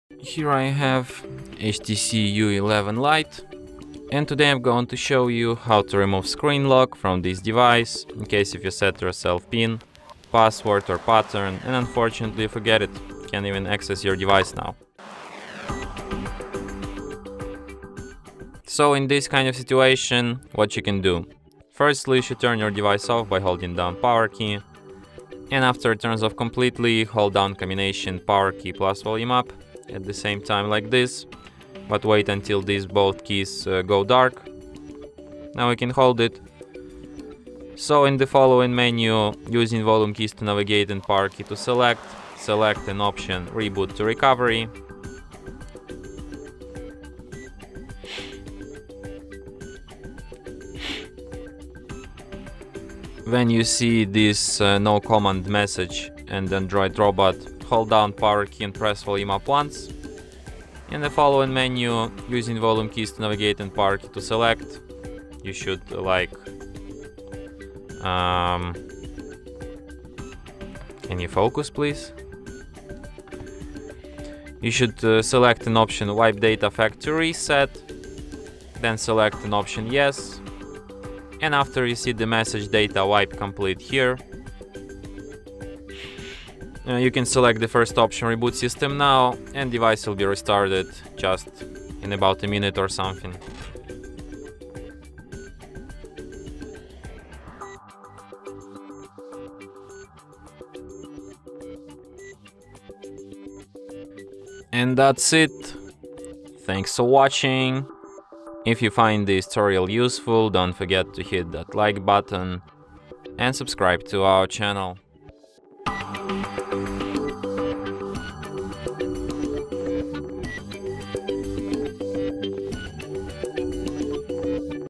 How to Hard Reset HTC U11 Life? | Wipe Data via Recovery Menu
Find out more:
This tutorial offers a comprehensive walkthrough on executing a hard reset on the HTC U11 Life by utilizing the Recovery Menu to wipe data. Whether you're encountering persistent software issues, seeking to restore your device to its default settings, or preparing it for a new user, this guide provides detailed instructions on performing a hard reset safely. From accessing the Recovery Menu to navigating the options and wiping data securely, this tutorial equips users with the knowledge needed to execute a hard reset effectively without compromising essential data.

How does a hard reset via the Recovery Menu differ from a standard factory reset?
What steps are involved in accessing the Recovery Menu on the HTC U11 Life?
Can you explain the precautions to take before performing a hard reset using this method?
What happens to installed apps and user data when executing a hard reset via the Recovery Menu?
Is there a specific way to back up data before performing a hard reset through the Recovery Menu?